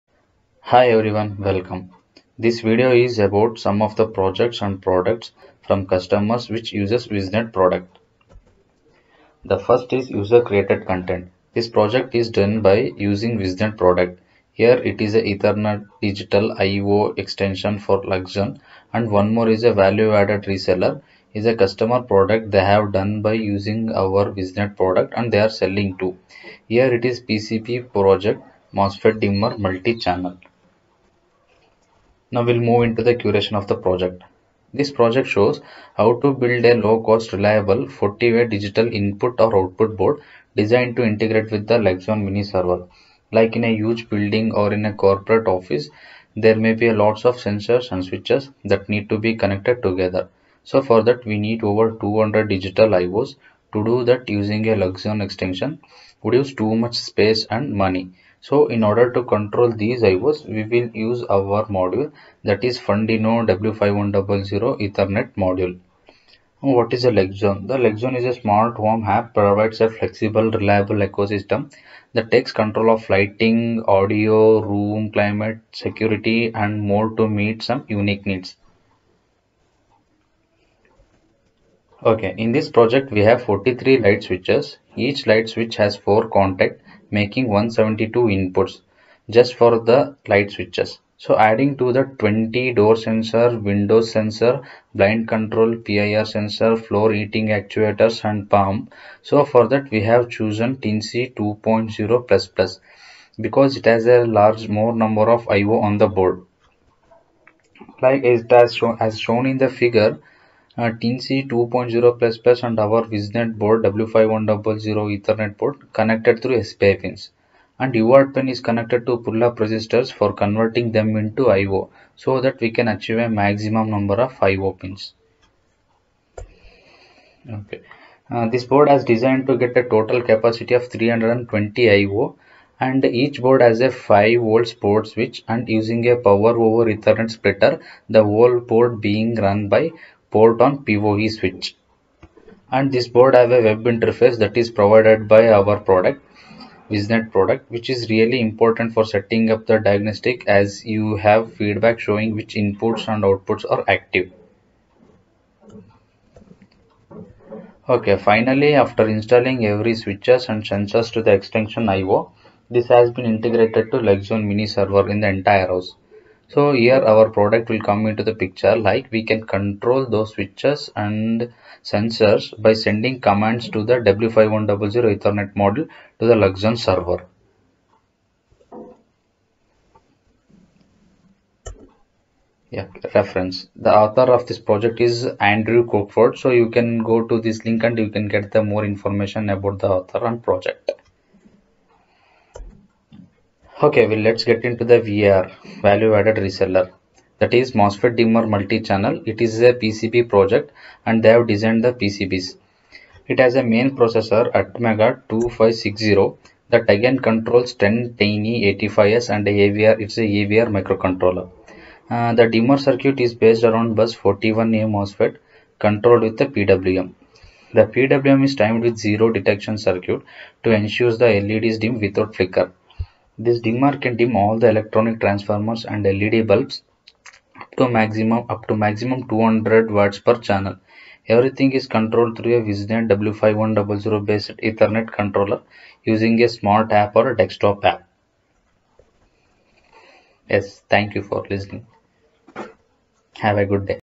Curated content of Wiznet products
This Video is about some of the projects and products from global customers which uses Wiznet Products.